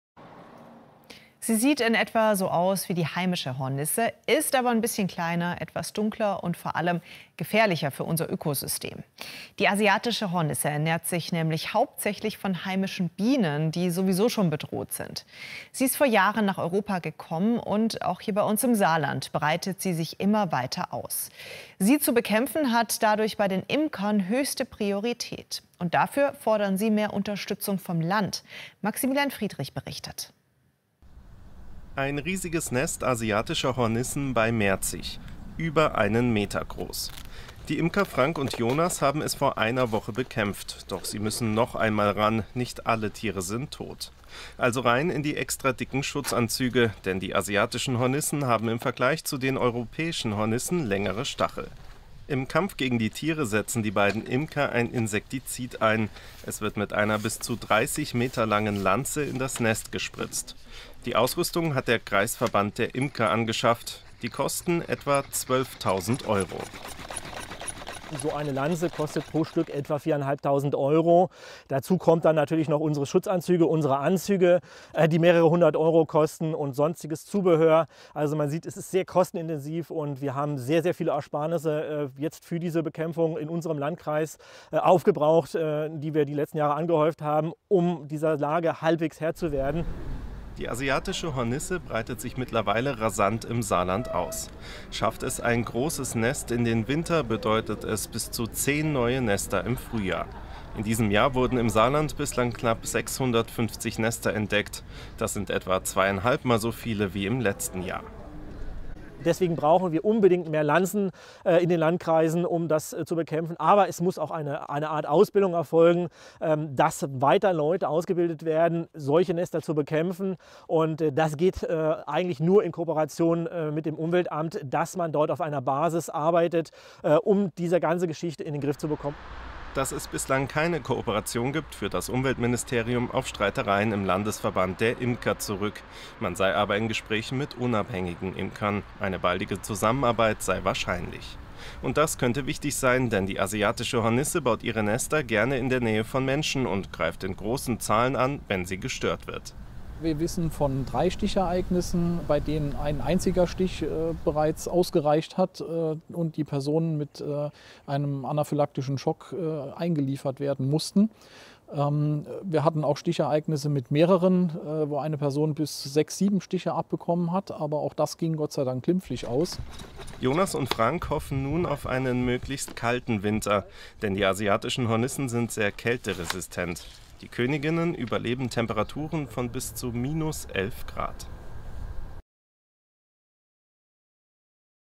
Vespa Velutina - Die Macher im Saarland - SR-Bericht 07.Nov 2024
Hallo Natur- und Bienenfreunde,
Sonschdwo wird geschwätzt, hier wird gemacht ;-)
Und das ehrenamtlich viele tausend Stunden pro Jahr, für die Biodiversität, für den Bürger und für den Schutz der Nutztierhaltung. Weit über 600 Nestentfernungen in 2024 (Stand Nov24). Dank Sichtungsmeldungen vieler aufgeklärter Bürger auf zentralem Meldeportal und dank einem flächendeckenden Netzwerk engagierter Imker. Die Arbeit der geschulten Helfer umfasst die Begutachtung der gemeldeten Spezies, die Beratung der Bürger, den Schutz heimischer Spezies, die aufwendige Nestlokalisierung, die Nesteindämmung und die Weiterbildung der Imkerschaft zur Anpassung der Betriebsweise an die hier nun etablierte Vespa Velutina.
Lediglich die Unterstützung des Landes hat durchaus noch Optimierungspotenzial.
Viele Grüße
Dirk

Quelle: Bericht des Saarländischen Rundfunks vom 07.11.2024
Weiteres im Printformat

Danke

HINWEIS
Alle wildlebenden Tiere sind durch das Bundesnaturschutzgesetz
und die Bundesartenschutzverordnung geschützt.
Manche wie die heimische Hornisse (vespa crabro) in besonderem Maße
Zu diesem Zeitpunkt im Jahr 2024 ist die Asiatische Hornisse
(vespa velutina) in der EU als invasive Art eingestuft.
Zum Schutz der heimischen Biodiversität durch zu starken
 exponentiellen Anstieg werden vorübergehend Nester
ausschließlich der vespa velutina beseitigt.
Was morgen ist, wird man sehen.
Grüße
Dirk

Ab 2025 als etablierte invasive Art nach Artikel 19 EU-Verordnung (EU) Nr. 1143 /2014 und folglich Behandlung nach einem Management- und Maßnahmenplan